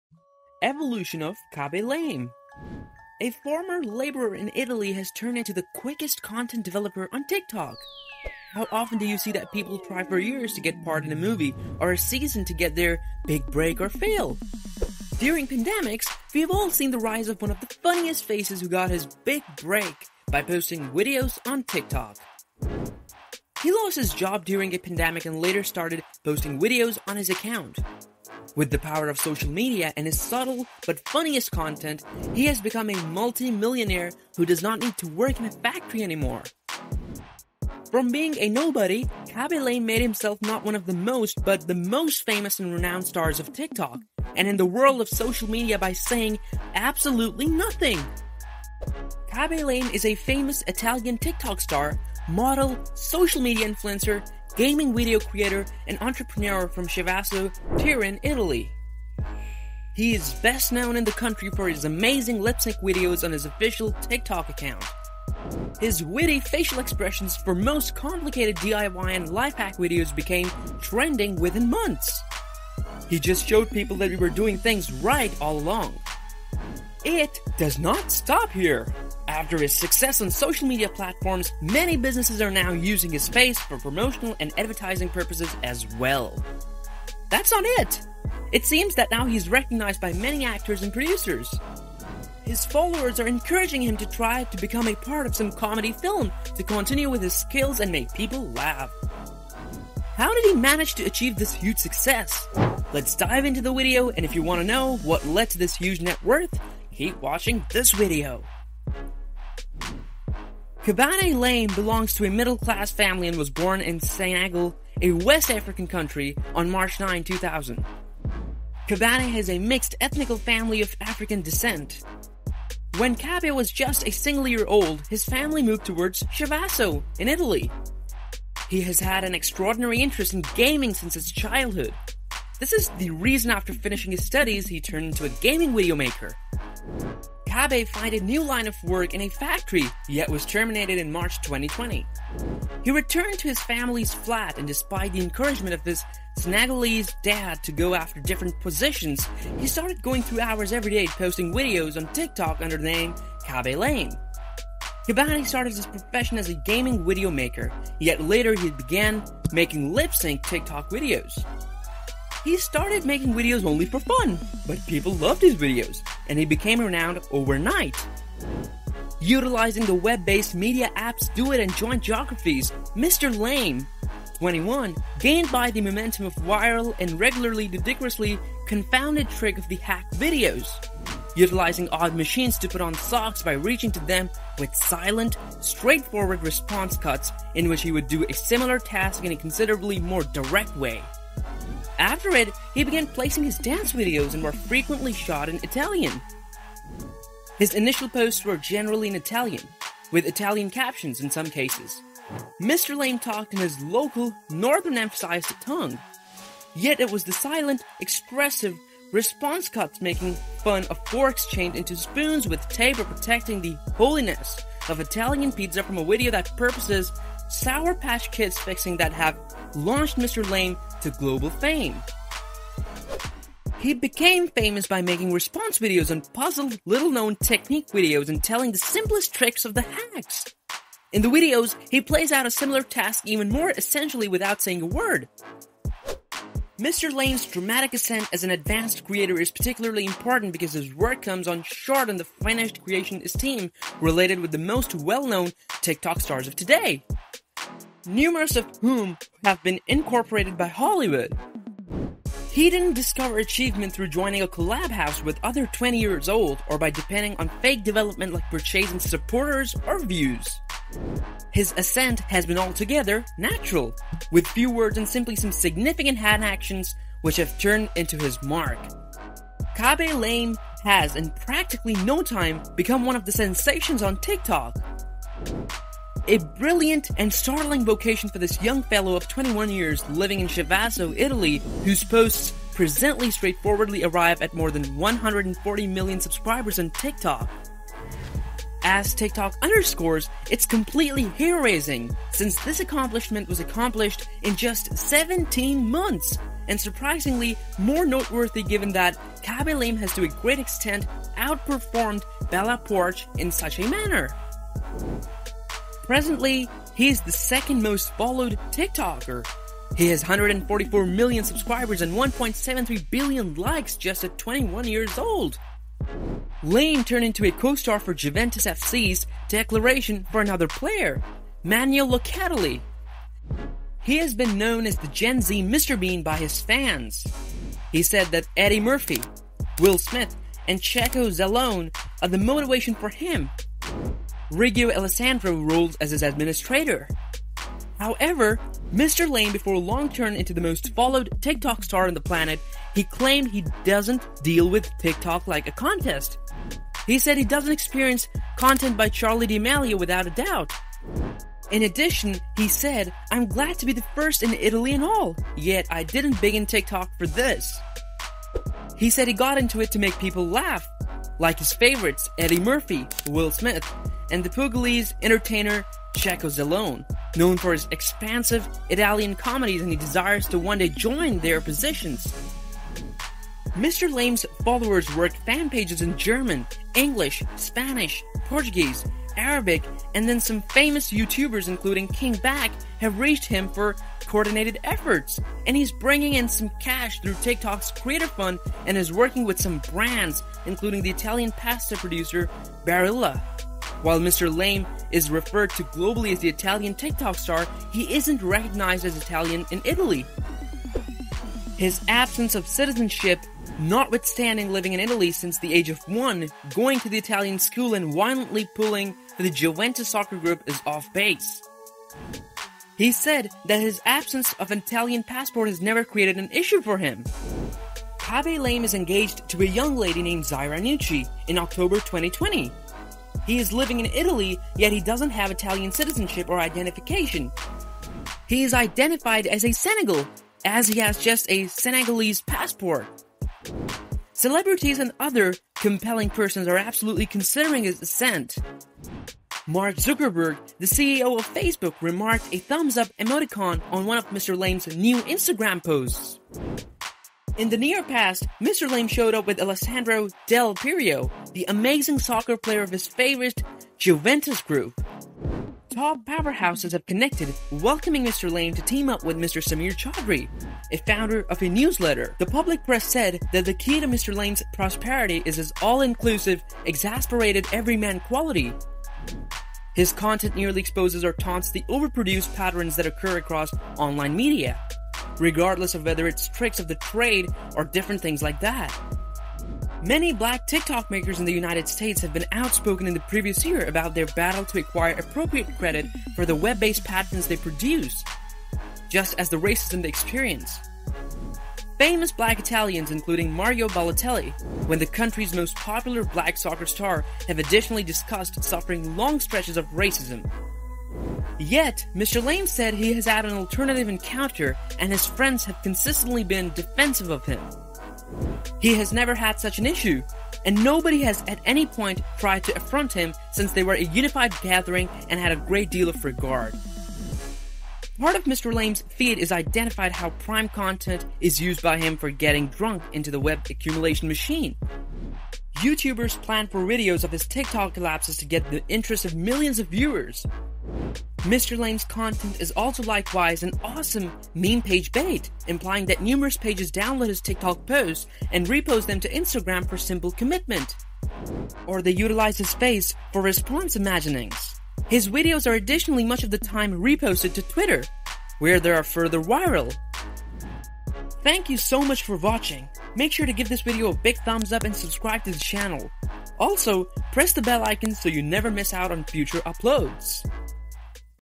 Evolution of Khaby Lame (Full Documentary) Zero to Hero Khaby Lame 💯👨
A former laborer in Italy has turned into the quickest content developer on TikTok. How often do you see that people try for years to get part in a movie or a season to get their big break and fail? During pandemics, we all have seen the rise of one of the funniest faces who got his “big break” by posting videos on Tik Tok. He lost his job during a pandemic and later started posting videos on his account. With the power of social media and his subtle but funniest content, he has become a multi-millionaire who does not need to work in a factory anymore.
From being a nobody, Khaby Lame made himself not one of the most but the most famous and renowned stars of Tik Tok and in the world of social media by saying absolutely, “nothing!” Khaby lame is a famous Italian TikTok star, model, social media influencer, gaming video creator, and entrepreneur from Chivasso, Turin, Italy. He is best known in the country for his amazing funny lip-sync videos on his official TikTok account.
His witty facial expressions for most complicated DIY and life hacks videos became trending within months. He just showed people that we were doing things right all along. It does not stop here. After his success on social media platforms, many businesses are now using his face for promotional and advertising purposes as well. That’s not it!!! It seems that he is now recognized by many actors and producers. His followers are encouraging him to try to become a part of some comedy film to continue with his skills and make people laugh.  How did he manage to achieve this huge success? Let’s dive into the video and if you wanna know what led to this huge net worth, keep watching the video!